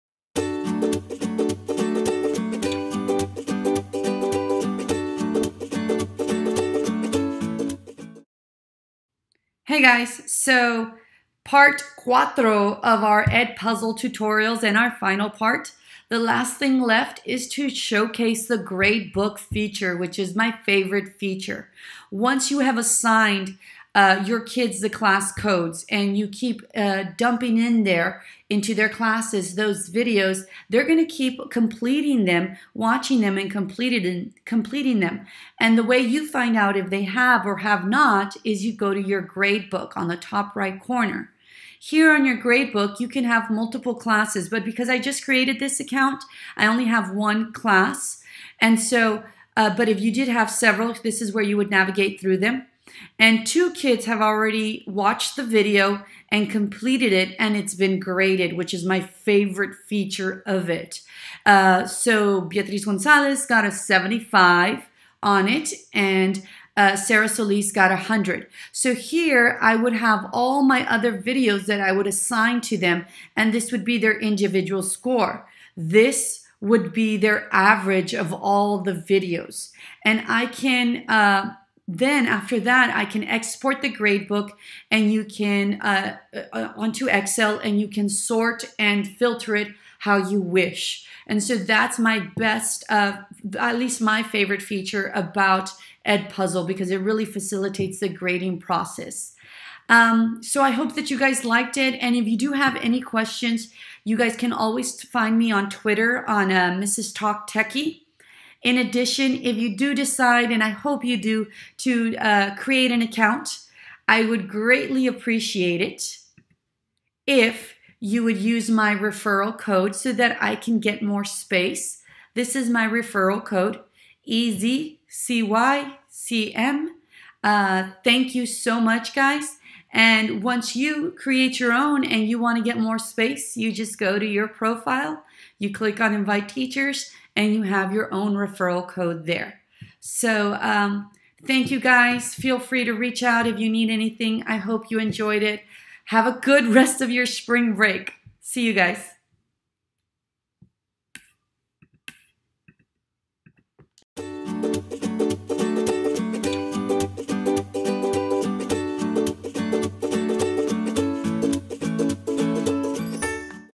EdPuzzle Pt4: Gradebook and referral code
In this last video, you get to see the gradebook feature and how to find your referral code for other teachers to use.

This video will go over how to create your own digital content by screen recording your iPad, uploading it to Youtube and finally adding it to edpuzzle!